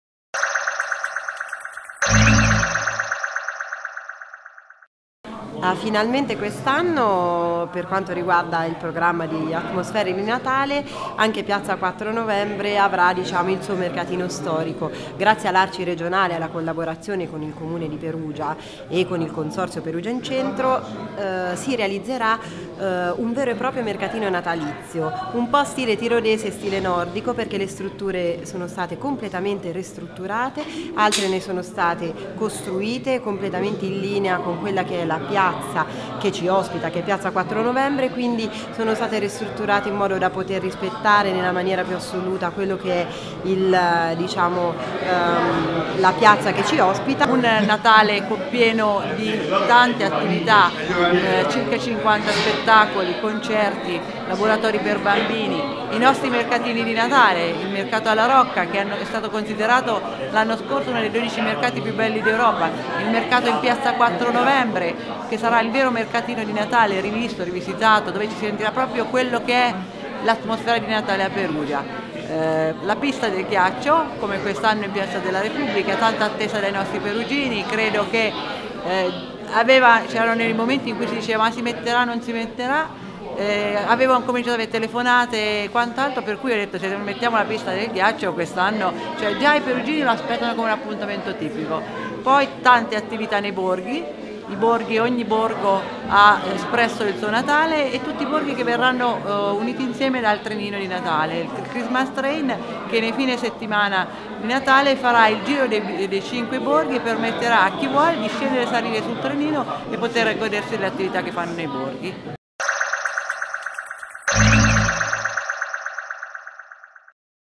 Perugia città del Natale: mercatini, presepi ed eventi in tutta la città
E' stato presentato il cartellone di eventi e di iniziative per il Natale a Perugia. Quest'anno c'è il Christmas train per piazze e borghi. Servizio Maurizio Troccoli